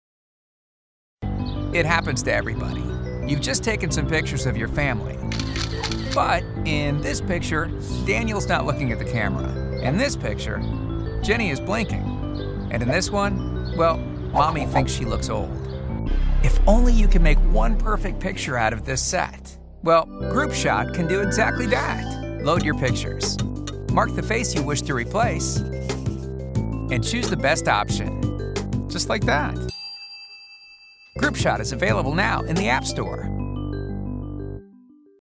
Groupshot -- amazing new photo app!
This app can fix any group shot where usually somebody is looking the other way or caught with an ugly face, a finger operated Photoshop studio with 3 touches in your mobile, incredible.